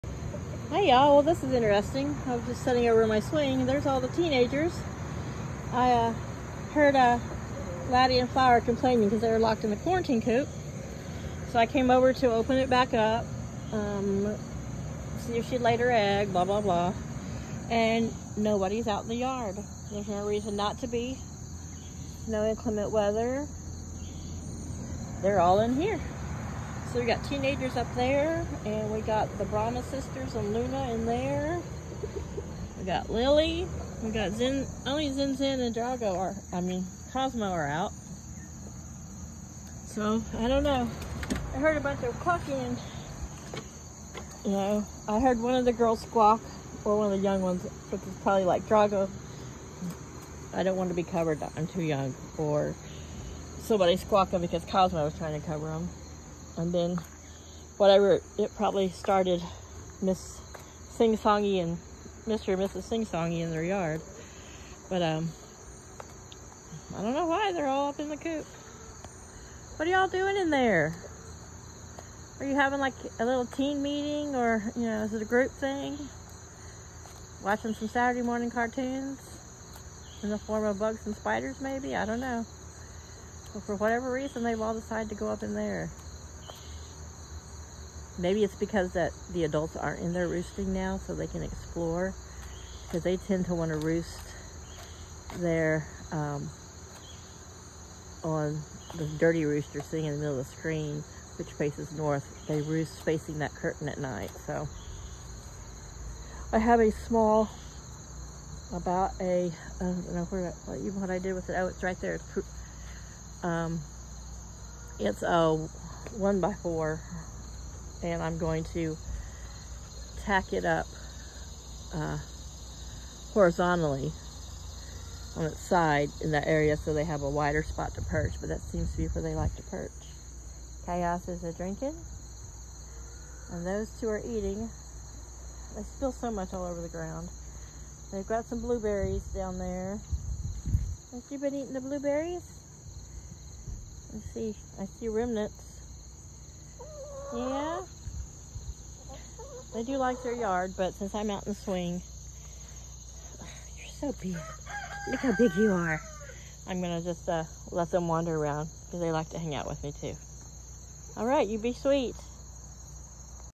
Teenage Chickens
Teen’s lounging about on. Saturday morning. Laddie with blueberry lips and tongue.